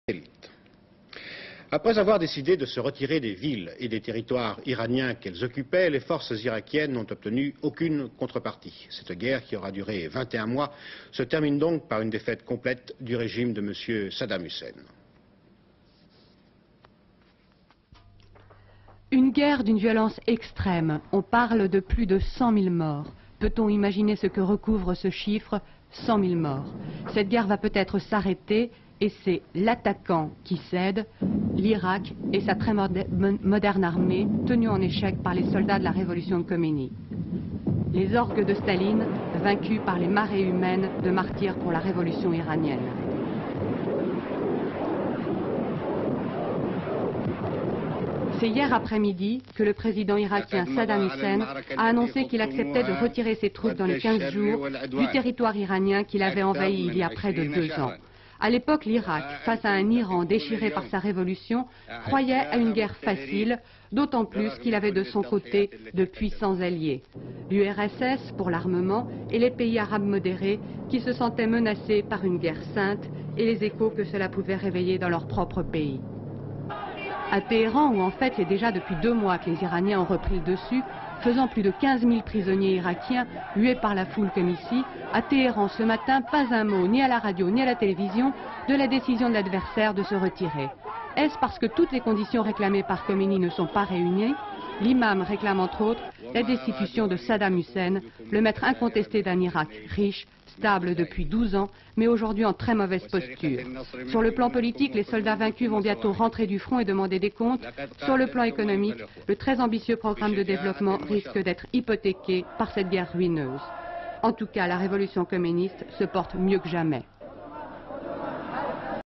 Guerre Irak-Iran
21 juin 1982
Bilan de la guerre Irak-Iran qui se solde par une défaite du régime de Saddam HUSSEIN avec plus de 100 000 morts. Images d'archive INA